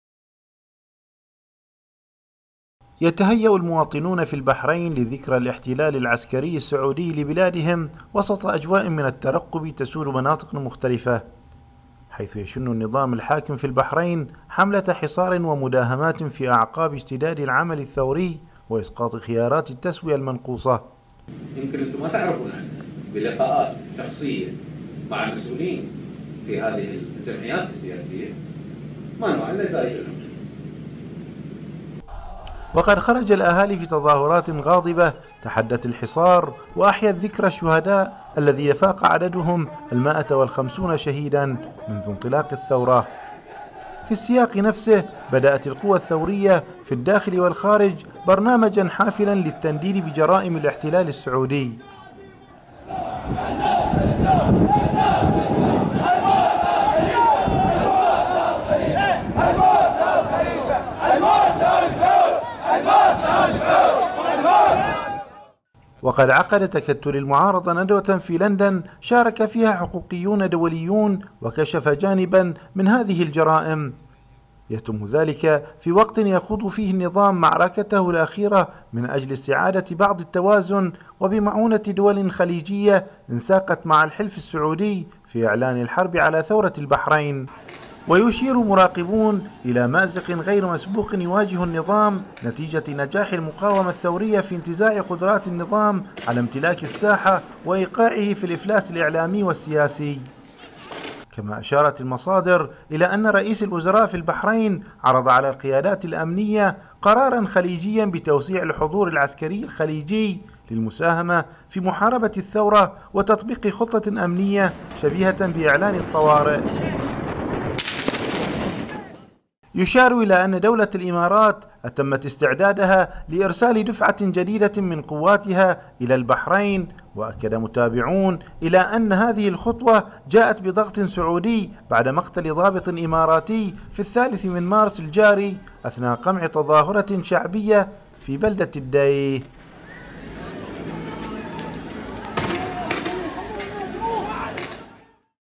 وكالة أنباء البحرين اليوم | 13 مارس 2014 | السلطات تعزز قبضتها الأمنية وأنباء عن تعزيزات خليجية
يتهيّأ المواطنون في البحرين لذكرى الإحتلال العسكري السّعودي لبلادهم وسط أجواءٍ من التّرقّب تسود مناطقَ مختلفة، حيث يشنّ النّظامُ الحاكم في البحرين حملةَ حصارٍ ومداهماتٍ في أعقابِ اشتدادِ العمل الثّوري وإسقاط خياراتِ التّسوية المنقوصة، وقد خرج الأهالي في تظاهراتٍ غاضبة تحدّت الحصارَ وأحيت ذكرى الشّهداء الذين بلغوا ١٥٦ شهيداً منذ انطلاق الثّورة.

في الاتجاهِ نفسه، بدأت القوى الثّوريّة في الدّاخل والخارج برنامجاً حافلاً للتّنديدِ بجرائم الاحتلال السّعودي، وقد عقد تكتّلُ المعارضة ندوةً في لندن شاركَ فيها حقوقيّون دوليّون وكشفت جانباً من هذه الجرائم.
يتمّ ذلك في وقتٍ يخوضُ فيه النّظامُ معركته الأخيرةَ من أجلِ استعادةِ بعضِ التّوازن وبمعونةٍ دولٍ خليجيّة انساقت مع الحلف السّعودي في إعلان الحربِ على ثورة البحرين، ويشيرُ مراقبون إلى مأزقٍ غير مسبوقٍ يواجهه النّظام نتيجةَ نجاحِ المقاومةِ الثّوريّةِ في انتزاعِ قدراتِ النّظامِ على امتلاكِ السّاحةِ وإيقاعه في الإفلاس الإعلامي والسّياسي.

كما أشارت المصادر إلى أنّ رئيس الوزراء في البحرين عرضَ على القياداتِ الأمنيّة قراراً خليجيّاً بتوسيعِ الحضور العسكريّ الخليجي للمساهمةِ في محاربةِ الثّورةِ وتطبيقِ خطّةٍ أمنيّةٍ شبيهةٍ بإعلان الطّوارئ. يُشارُ إلى أنّ دولة الإمارات أتمّت استعدادها لإرسالِ دفعةٍ جديدةٍ من قوّاتها إلى البحرين، وأكّد متابعون إلى أنّ هذه الخطوة جاءت بضغطٍ سعوديّ بعد مقتلِ ضابط إماراتي في الثالث من مارس الجاري أثناء قمْع تظاهرةٍ شعبيّة في بلدة الدّيه.